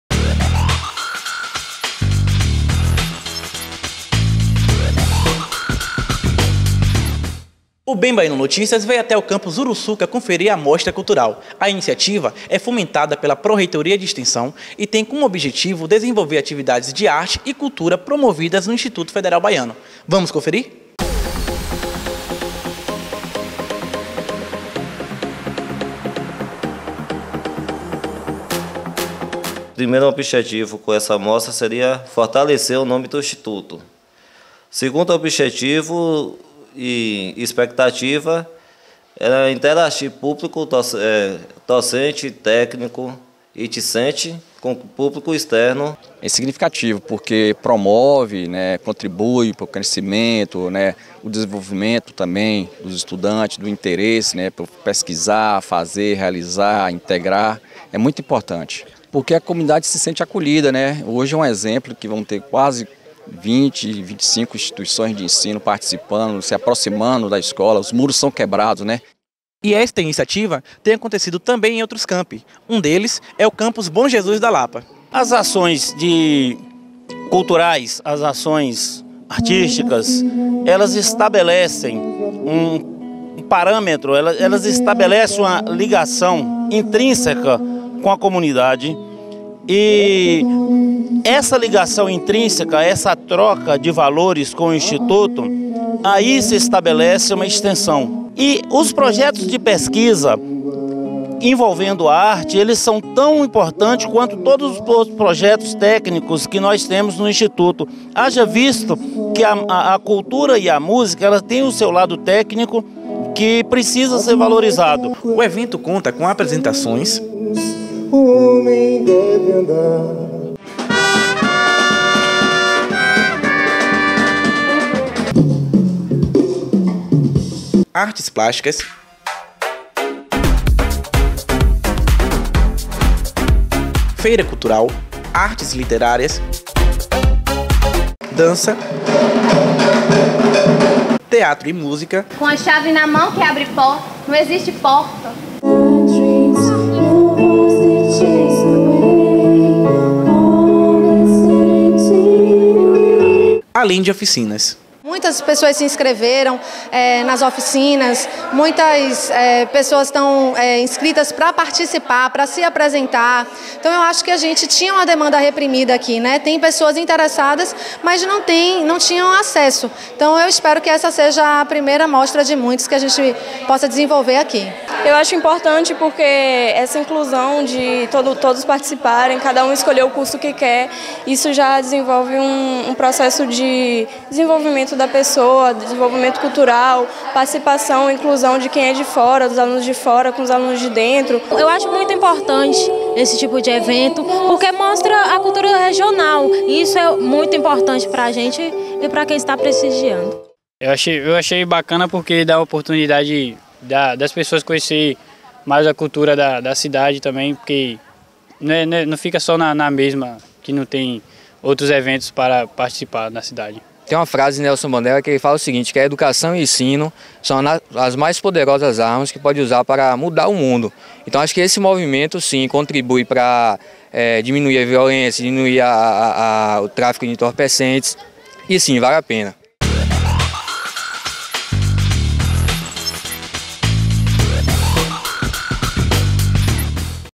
BEM BAIANO NOTÍCIAS - Mostra Cultural IF Baiano
Nesta edição do Bem Baiano Notícias, vamos conferir o evento ocorrido nas cidades de Uruçuca e Bom Jesus da Lapa, no campus do Instituto Federal de Educação, Ciência e Tecnologia Baiano.